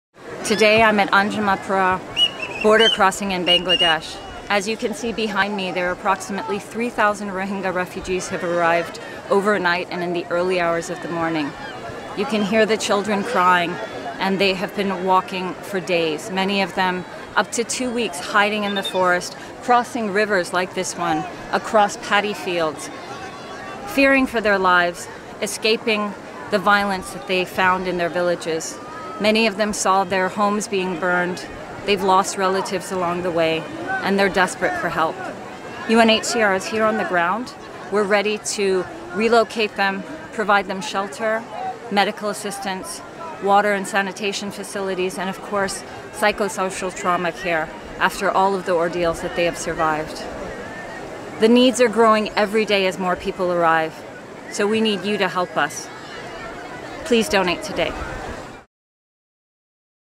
孟加拉 羅興亞難民危機
在孟加拉邊境的Anjumanpara，現時逾千名羅興亞難民抵達。他們為了逃離家園的暴力踏上危機四伏的旅途，聯合國難民署目前在當地重新安置他們，提供庇護所、醫療援助、食水及衛生設施，並在其經歷所有苦難後提供心理創傷支援。我們急需你的支持，繼續為羅興亞難民提供支援。

你亦可透過下列途徑捐款 ：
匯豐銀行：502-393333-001